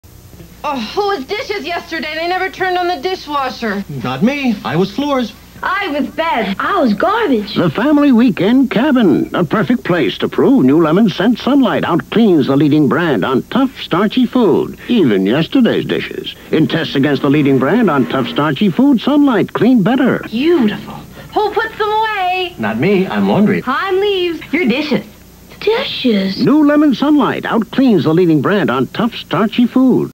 Sun Light dishwashing detergent commercial 1983
Commercial from a summer 1983 recording of Police Story and A Killing Affair on KPIX TV 5 San Francisco CBS